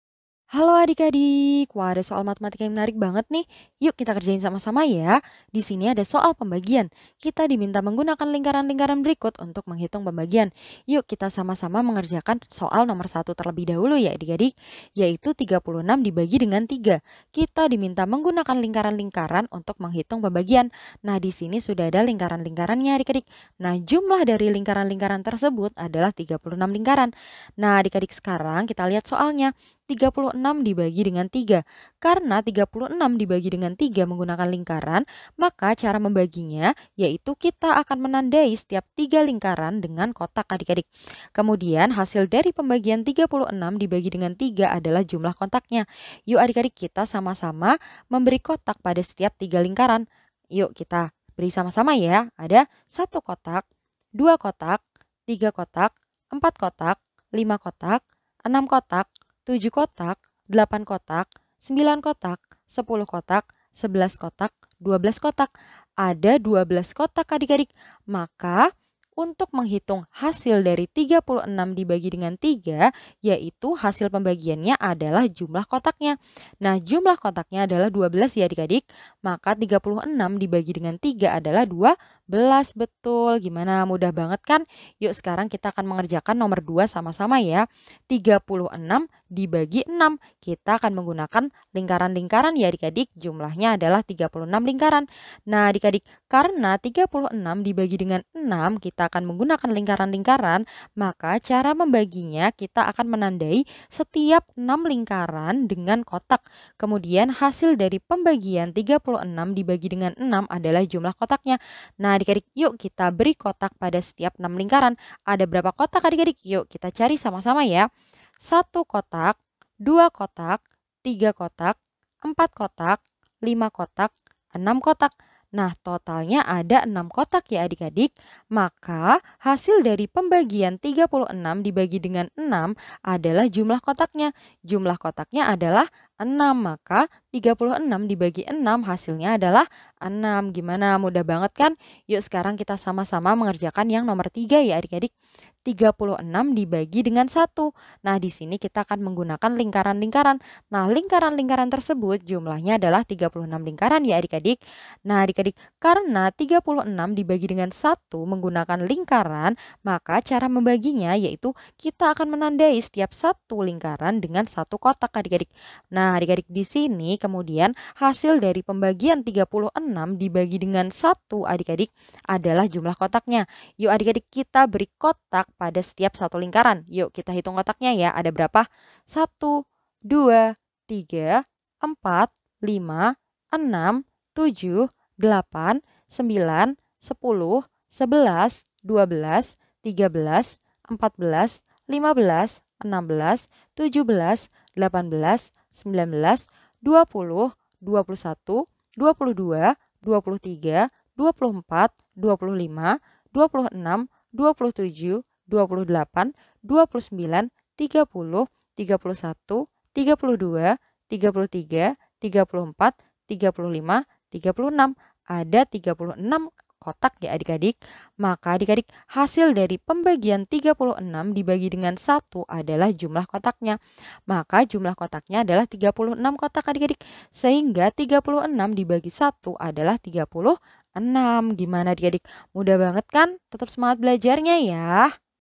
1. 36 : 3 = ... 2. 36 : 6 = ... 3. 36 : 1 =.

Topik atau Materi: Operasi Bilangan Cacah - Bilangan Cacah - Aritmatika - Matematika Kelas 3 SD - K-13 Revisi

Sumber Soal: Matematika Kelas 4, K-13 Revisi, DA, 2018 , Erlangga

00:00 : Bedah Soal
01:00 : Konsep, rumus dan pengertian Operasi Bilangan Cacah
01:18 : Langkah penyelesaian soal
02:55 : Jawaban akhir
03:25 : Penutup